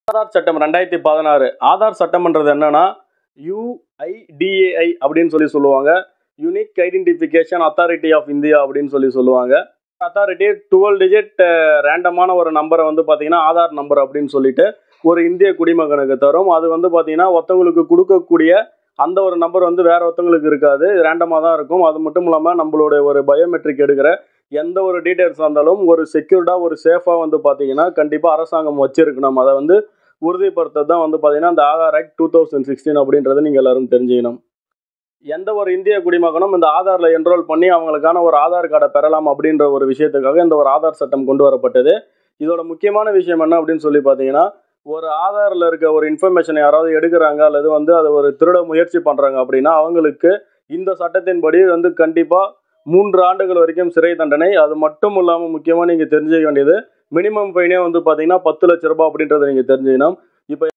Aadhar Card Law | Advocate Sudarsanan | Satta Maiyam
This video is about the Aadhar card act in india.